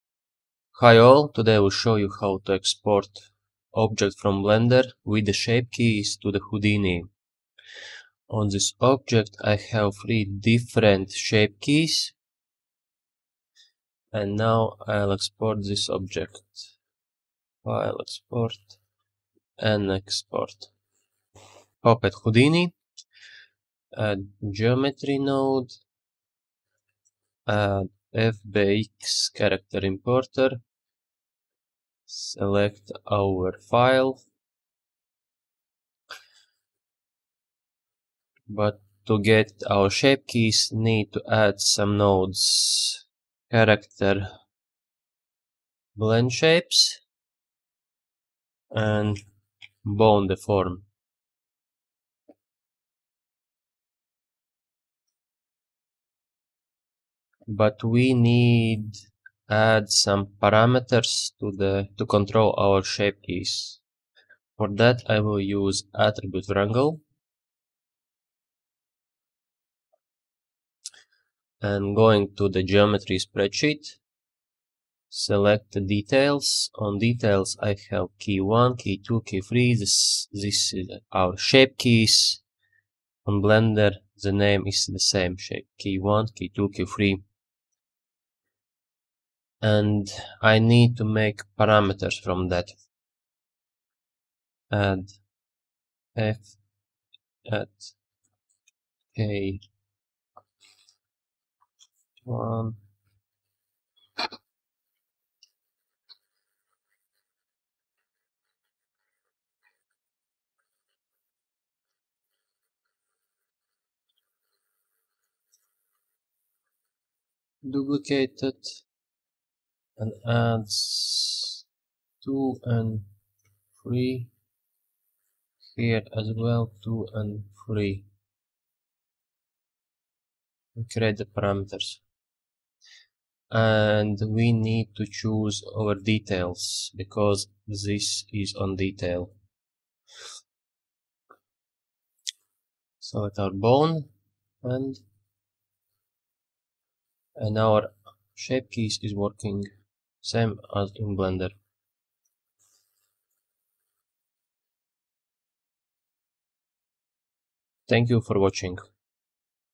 Blender Shape Keys inside Houdini
Workflow how to import Blender object with shape keys inside Houdini